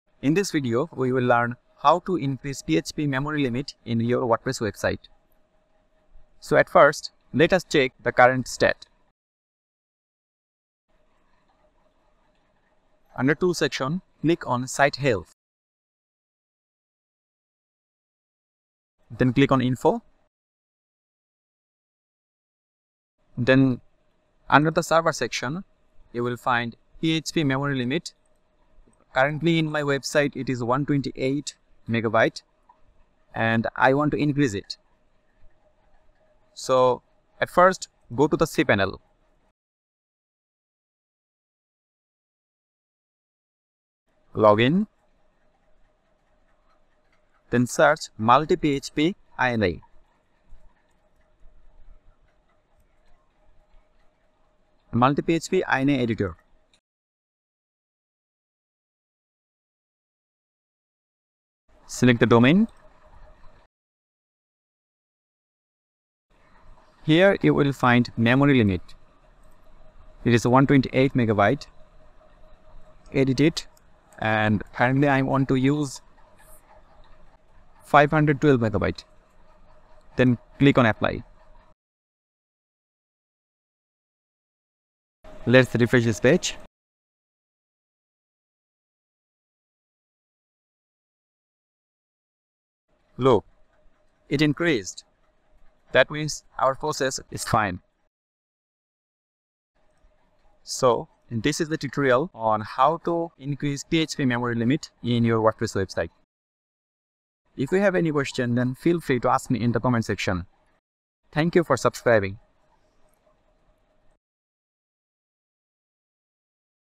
How to Increase PHP Memory Limit in WordPress (Using cPanel) | Quick & Easy Tutorial
Having trouble with memory limit issues in WordPress? In this step-by-step tutorial, I'll show you how to increase the PHP memory limit in WordPress using cPanel. If you're facing errors like "Allowed memory size exhausted," this guide will help you fix it in just a few minutes!

WordPress themes and plugins can sometimes require more memory than your default PHP settings provide. Increasing the PHP memory limit ensures your website runs smoothly and helps avoid unexpected crashes.

Whether you're a beginner or an experienced WordPress user, this tutorial is easy to follow.

(Cheapest on the planet 😱😱😱 )